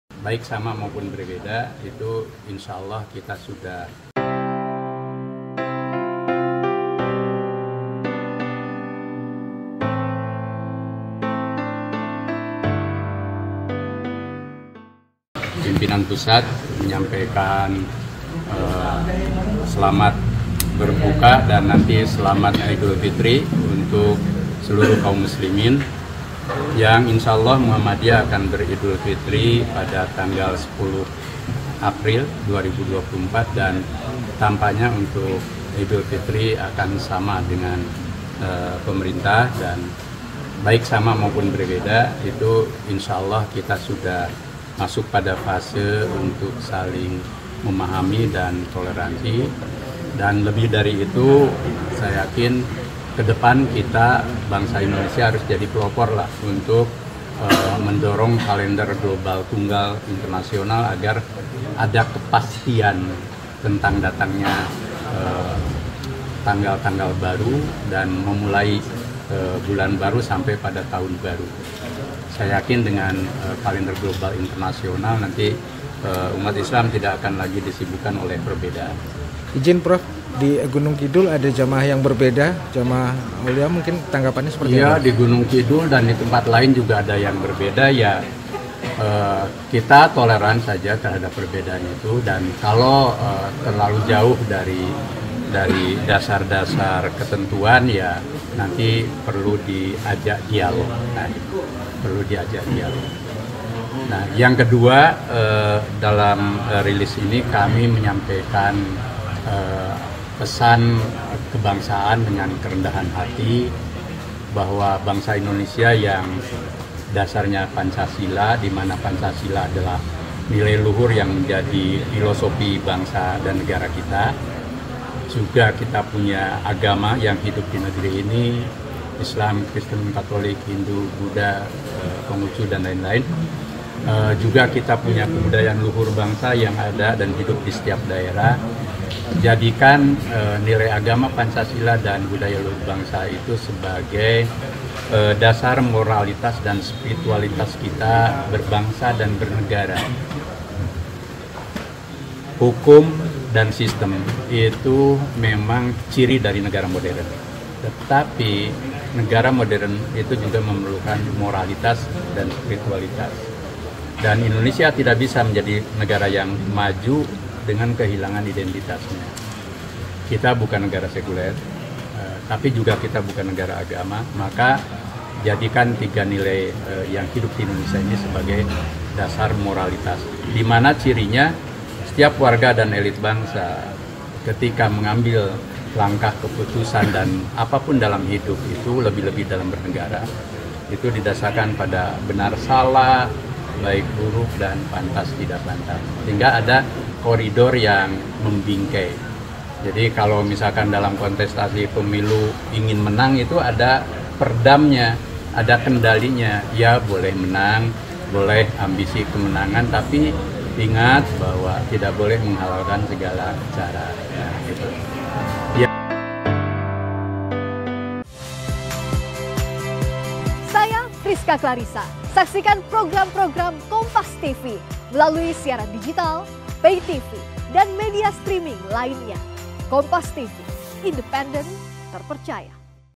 Jelang Idul Fitri 2024, Haedar Nashir Tekankan Pentingnya Saling Menghargai dan Bertoleransi!
YOGYAKARTA, KOMPAS.TV - Ketua Umum PP Muhammadiyah, Haedar Nashir meminta semua pihak untuk bisa bertoleransi jika nantinya terjadi perbedaan dalam penetapan Lebaran 2024.

"Selamat Idulfitri untuk seluruh kaum muslimin, yang insyaallah Muhammadiyah akan Idulfitri pada 10 April 2024," ujar Ketum PP Muhammadiyah, Haedar Nashir pada Selasa, (9/4/2024).

"Baik sama maupun berbeda, insyaallah kita masuk pada fase untuk saling memahami dan toleransi," lanjutnya.